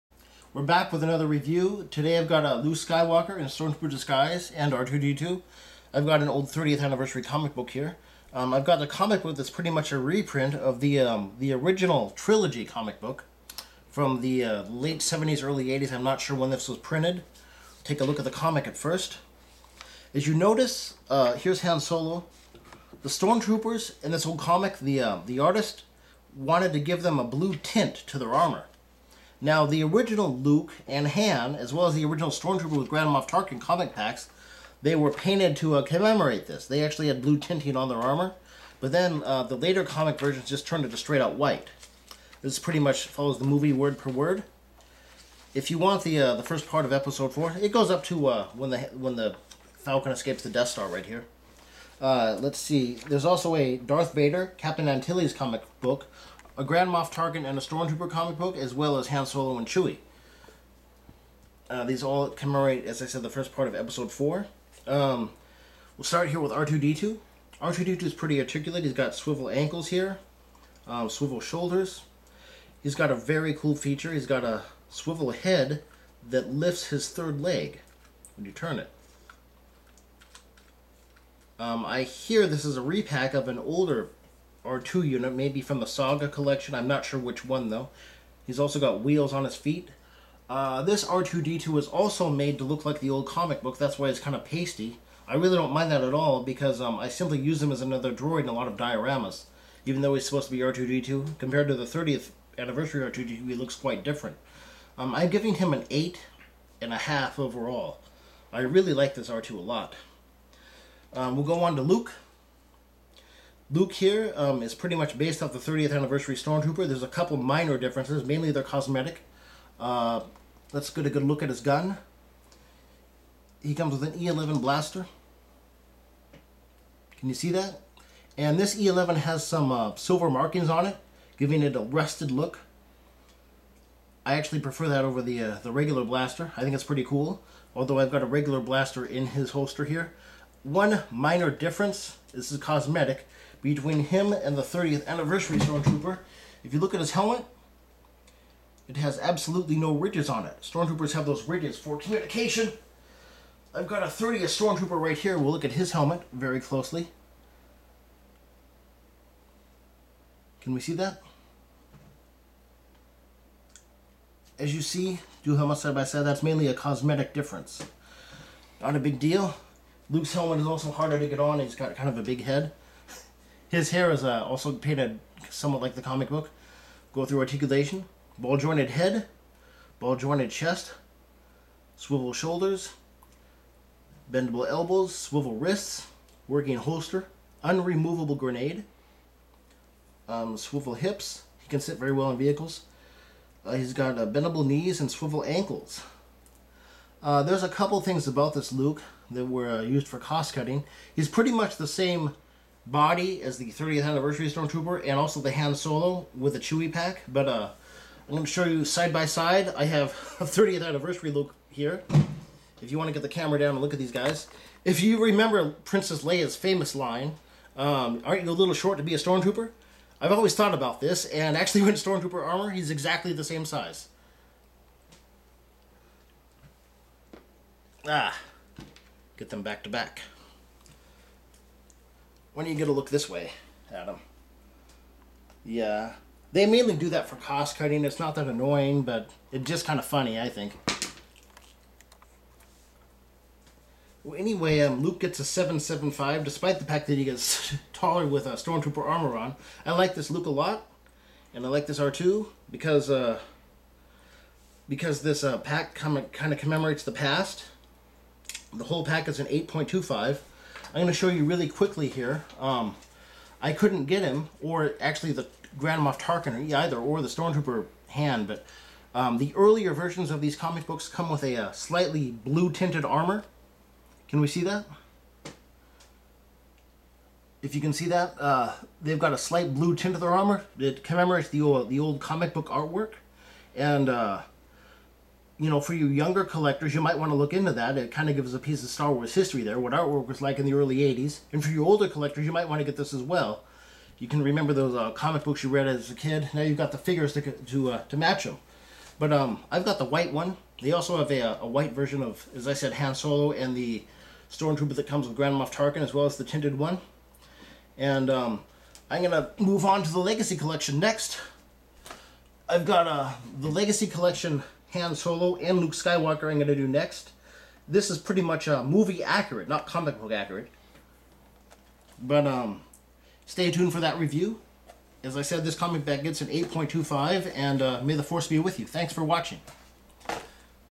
Star Wars Stormtrooper Luke & R2 D2 Comic Pack Review
Luke in Stormtrooper disguise and R2-D2 from the 30th anniv comic pack. Both figures are painted to commemerate the old comic book colers.